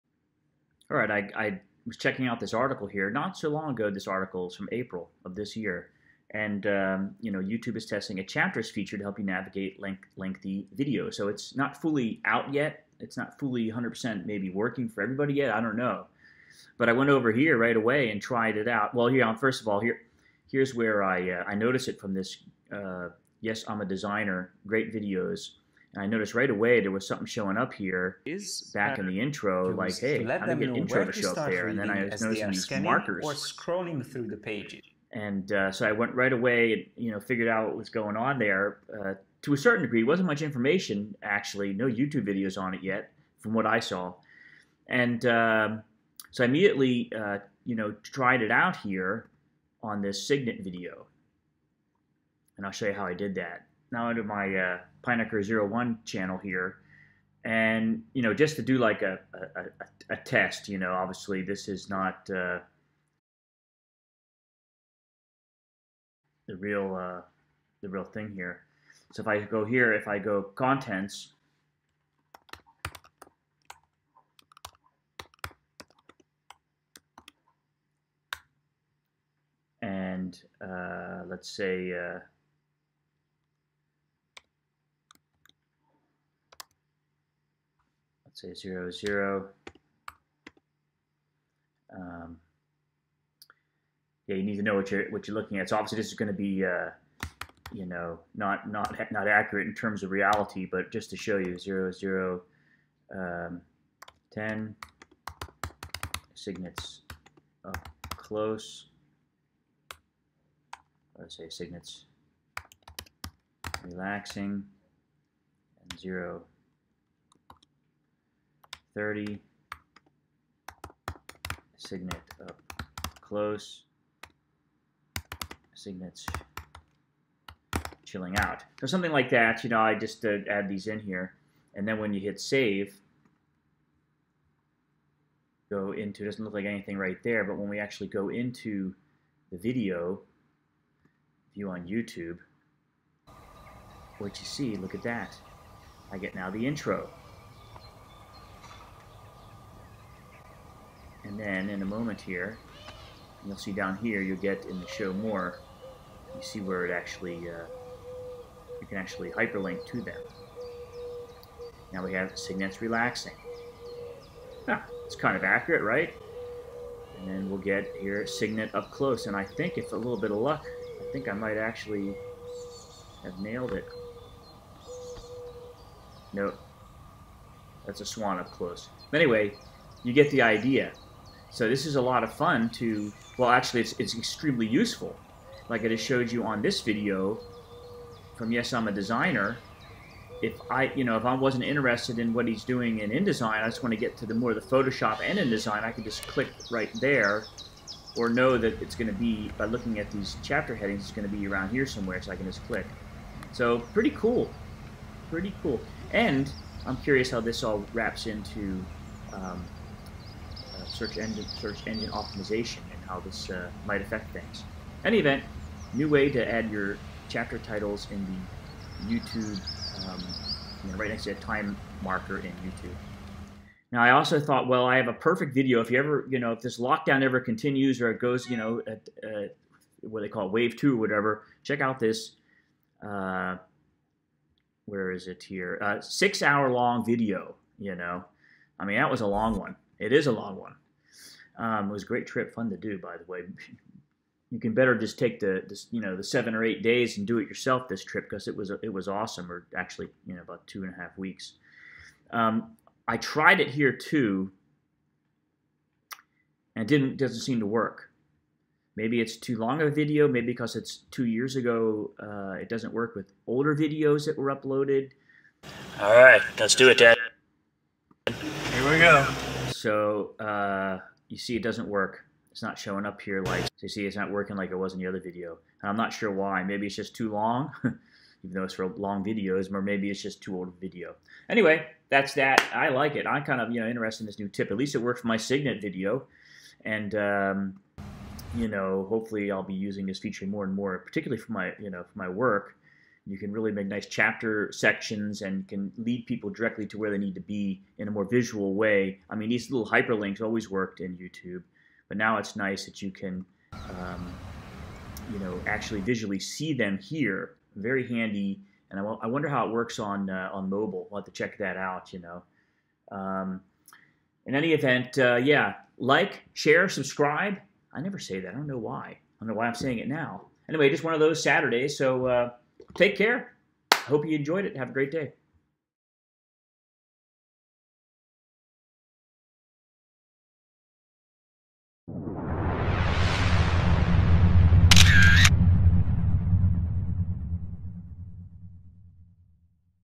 YouTube Feature - adding Chapter Markers in video progress bar
YouTube "Video Chapters"! Saw this new YouTube feature in practice today, Chapter markers or chapter timestamps but then also in the video progress bar… I will explain that all in this video and if it all works, as it should, you will see the chapter annotations or time hyperlinks from the description as sections broken up in the timeline bar or video progress section down below in the video viewing frame.

This graphic designer was kind enough to reply back to me, which I saw after I made the video, with:

Yes I'm a Designer
@Markzware It is a new feature, YouTube converts timestamps in description to chapters. Make sure to use zero:zerozero as first timestamp to make the feature wowor (I had to type out the zeros otherwise I couldn’t add that new feature in this video!)
CONTENTS:
0:00 - Intro via "The Next Web” article on new YouTube feature
0:21 - Yes I’m A Designer video example of Chapters
0:45 - How to set-up YouTube Chapter Markers or timestamps
2:15 - The results with timestamps to Chapter sections in video progress bar
3:00 - Why this new YouTube feature is useful
3:43 - New feature does not work on much older videos??
4:50 - What I will use Chapter markers for
5:15 - Closing - like, subscribe, etc - Thank you!

So very cool, all around! I’m curious if this soft release by YouTube of the chapter markers will help search engine optimisation (SEO) at all? In any event, very handy for long or technical videos. A great way to visually see when or where the next section starts.

YouTube feature to add chapter markers or segments to your video progress bar.